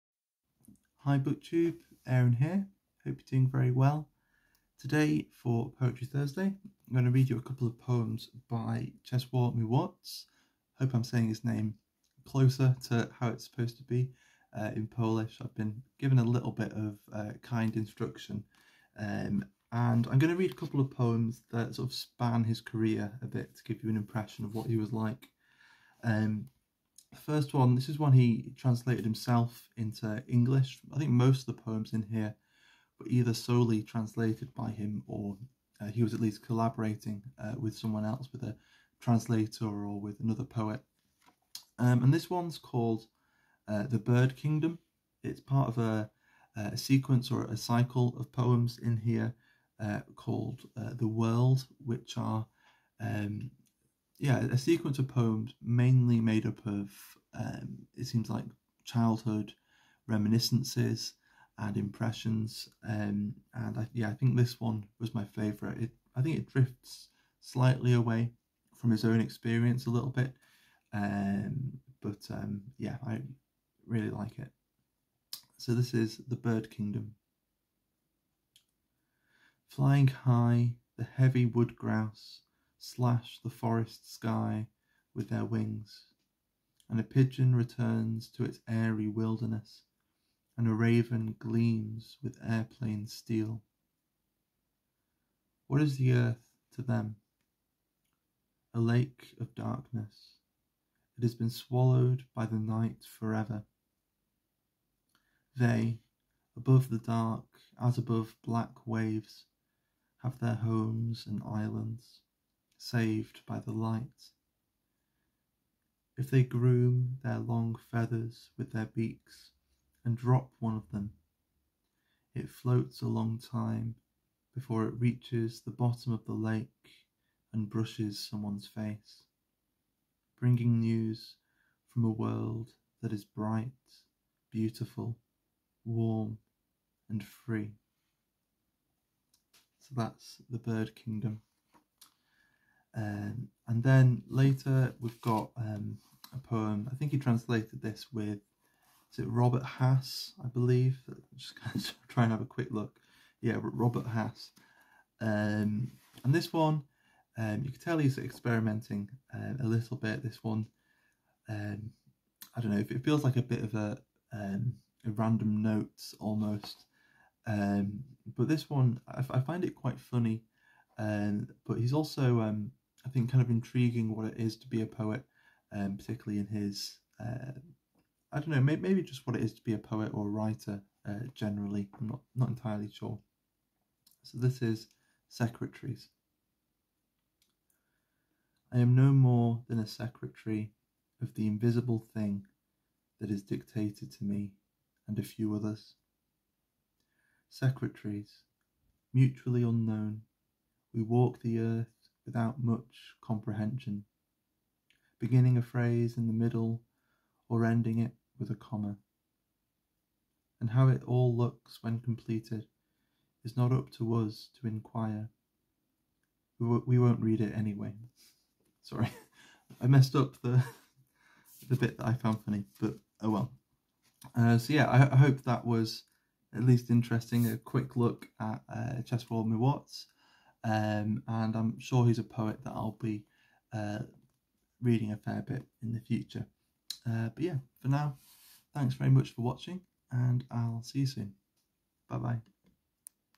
Poetry Thursday: Czesław Miłosz
In which I read 'The Bird Kingdom' and 'Secretaries' by Czesław Miłosz.

Voxer: afacer916